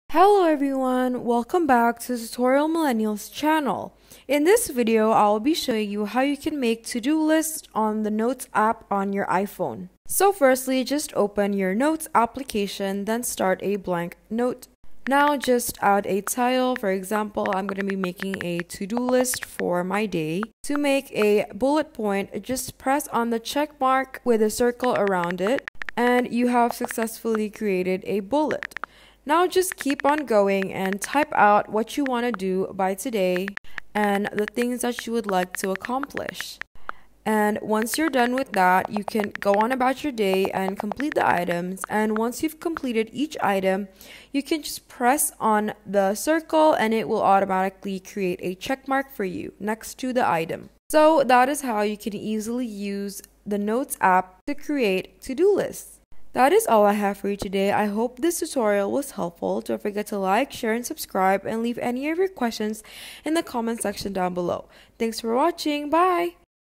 How to Create Bullet-Point To-Do Lists on iPhone Notes App
This is how you can create to do losts on your notes app on iPhone!